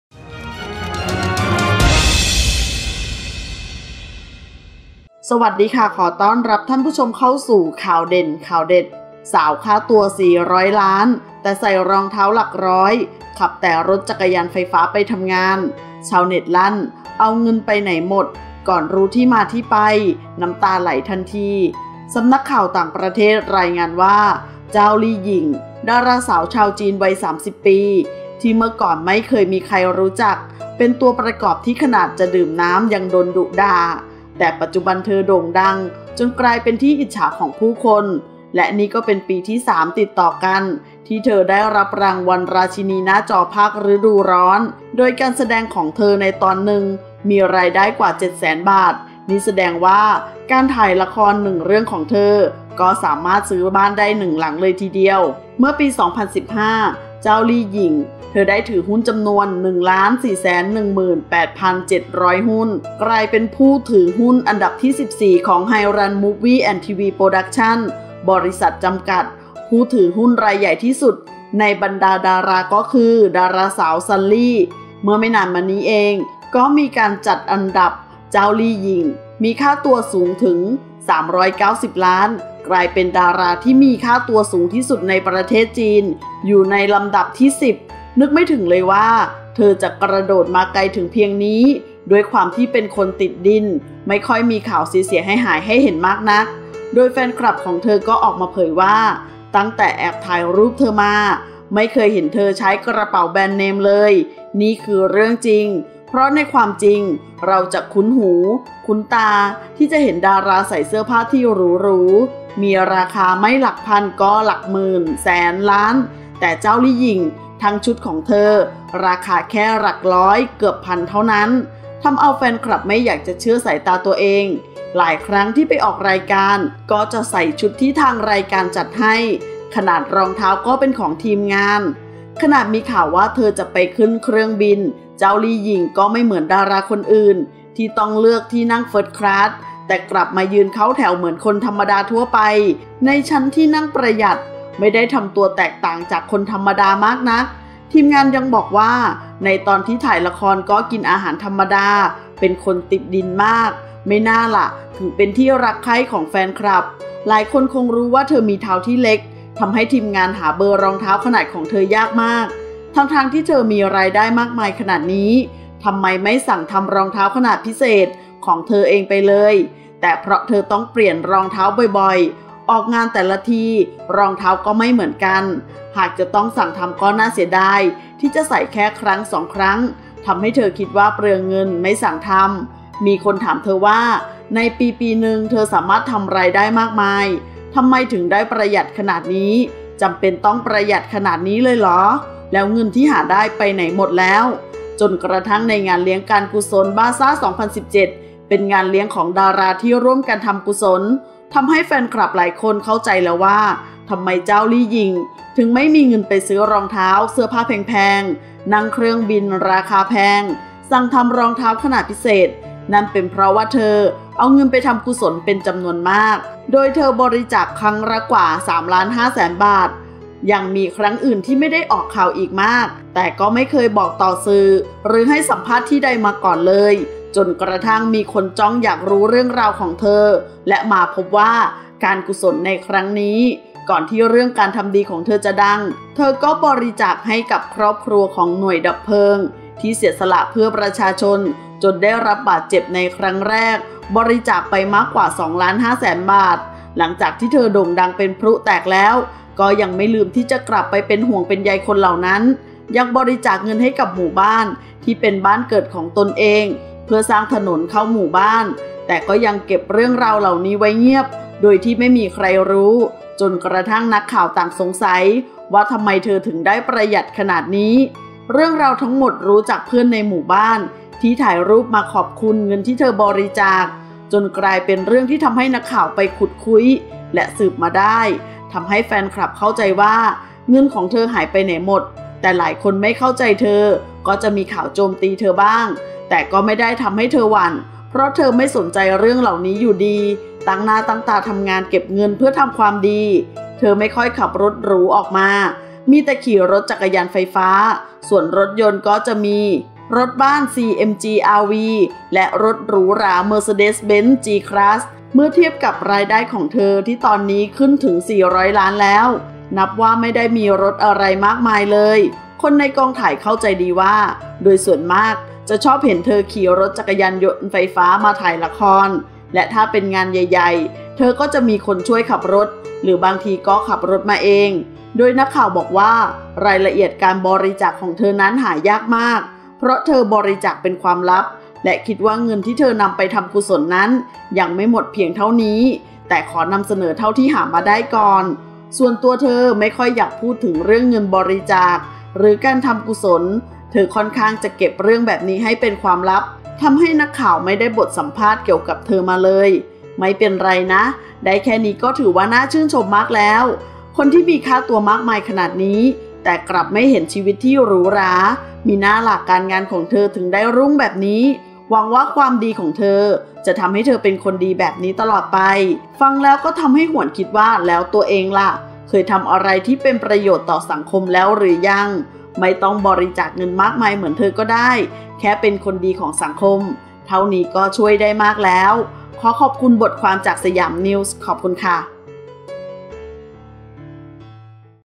สาวค่าตัว 400 ล้านแต่ใส่ รองเท้าหลักร้อย ขับแต่รถจักรยานไฟฟ้าไปทำงาน ชาวเน็ตลั่นเอาเงินไปไหนหมด
สาวค่าตัว 400 ล้านแต่ใส่ รองเท้าหลักร้อย ขับแต่รถจักรยานไฟฟ้าไปทำงาน ชาวเน็ตลั่นเอาเงินไปไหนหมด ก่อนรู้ที่มาที่ไป น้ำตาไหลทันที